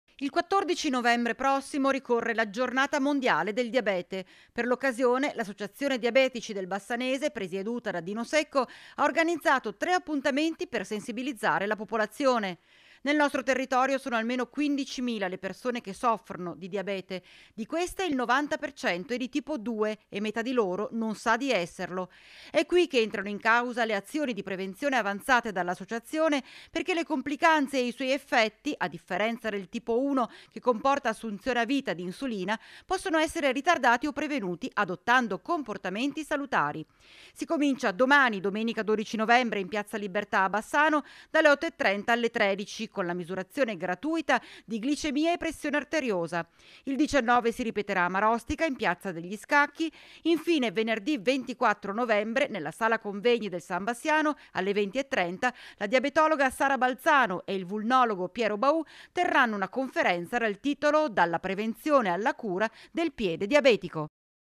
L'ASSOCIAZIONE DIABETICI DEL BASSANESE ORGANIZZA TRE INCONTRI PER PREVENIRE LA MALATTIA | 11/11/2023
TG Bassano del Grappa 11/11/2023 - BASSANO DEL GRAPPA - Da domani tre incontri per imparare a prevenire il diabete || Il 14 novembre prossimo ricorre la giornata mondiale del diabete. Per l'occasione l'associazione diabetici del bassanese, presieduta da Dino Secco, ha organizzato tre appuntamenti per sensibilizzare la popolazione.Nel nostro territorio sono almeno 15 mila le persone che soffrono di diabete.  Di queste il 90% è di tipo 2 e metà di loro non sa di esserlo. E' qui che entrano in causa le azioni di prevenzione avanzate dall'associazione, perchè le complicanze e i suoi effetti, a differenza del tipo 1 che comporta assunzione a vita di insulina, possono essere ritardati o prevenuti adottando comportamenti salutari.Si comincia domani, domenica 12 novembre, in Piazza Libertà a Bassano, dalle 8.30 alle 13, con la misurazione gratuita di glicemia e pressione arteriosa. Il 19 si ripeterà a Marostica, in piazza degli Scacchi. Infine venerdì 24 novembre, nella sala convegni del San Bassiano, alle 20.30, la diabetologa Sara Balzano e il vulnologo Piero Baù terrano una conferenza dal titolo "Dalla prevenzione alla cura del piede diabetico". (Servizio di Vanessa Banzato) - Segui Rete Veneta anche sul digitale terrestre! Visita il sito